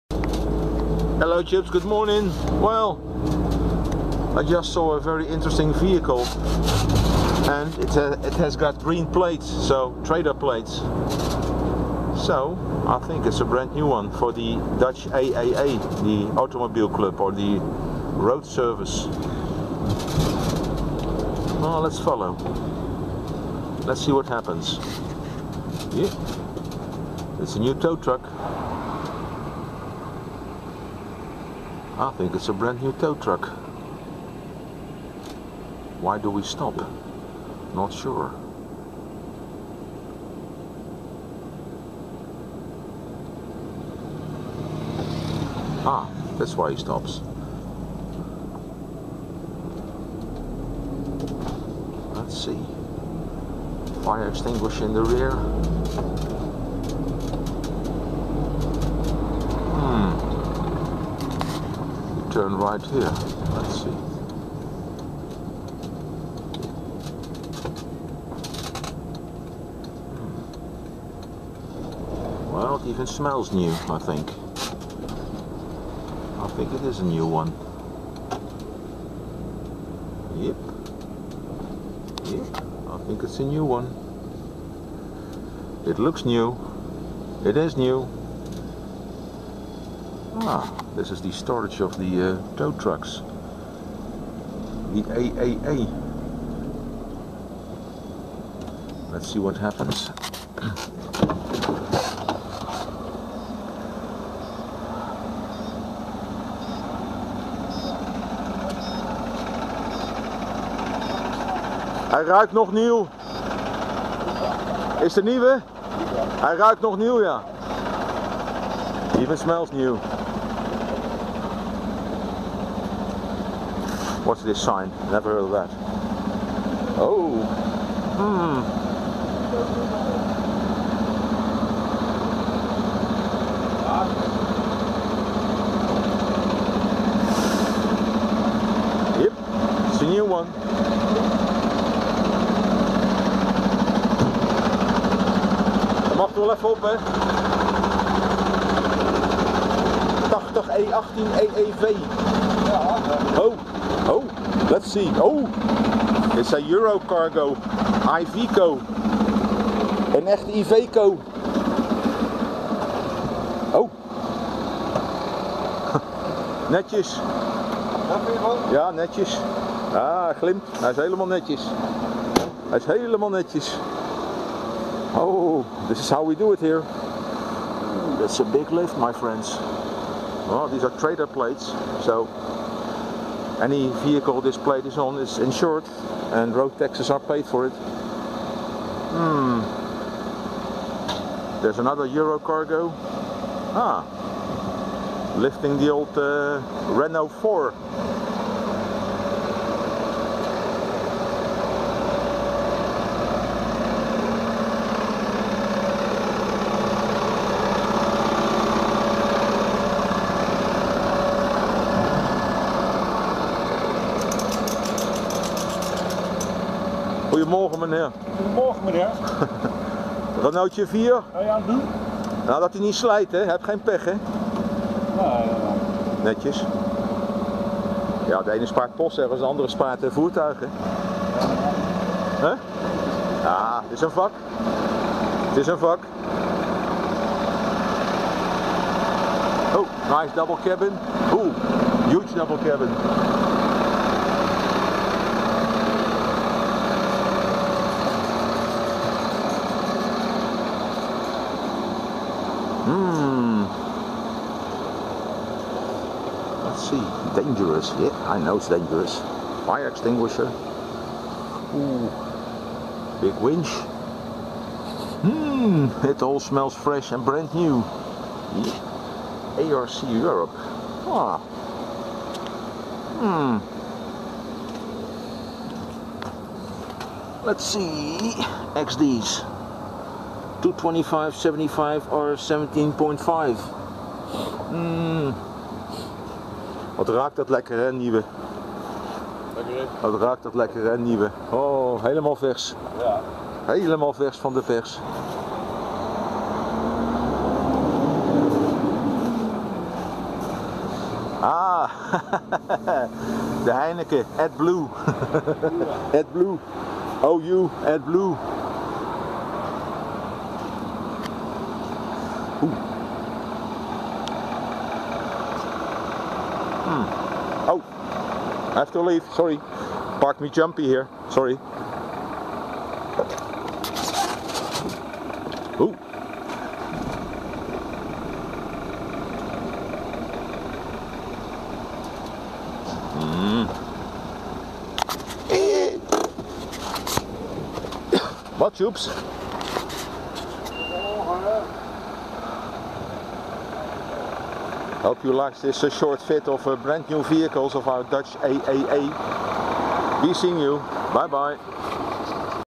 New Iveco 80E18 EEV towtruck AAA / Wegenwacht . ANWB
First testride of this 2011 Iveco Eurocargo towtruck for the Dutch AAA or Wegenwacht in Dutch., fitted wit the AdBlue system. , behind the other towtruck is a Renault 4L , used to patrol the highways here in the 70's.